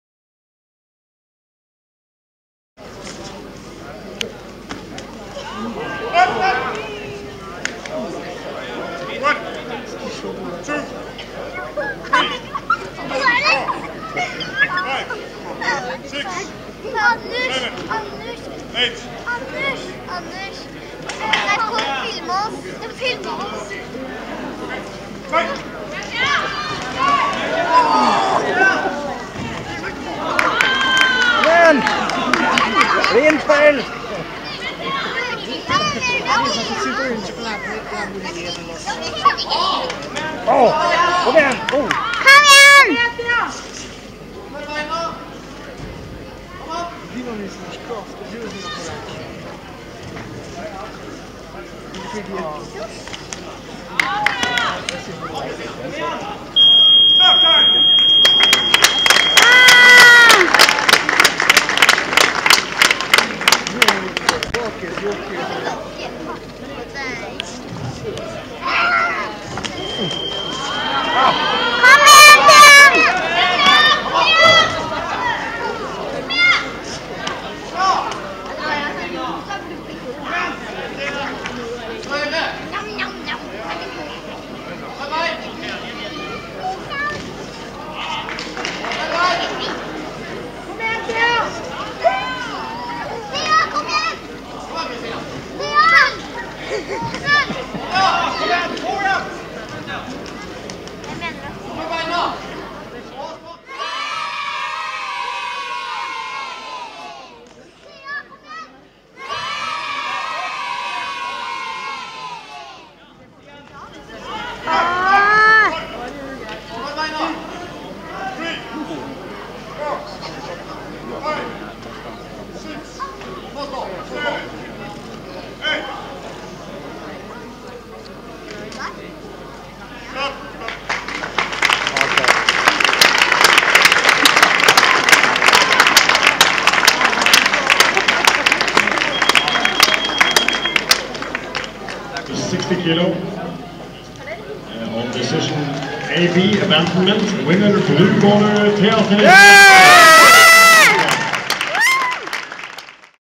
Scandinavian Open Kickboxing
Scandiavian Open 2012, Bjølsenhallen, Oslo. Thea Therese Næss. Seier på AB.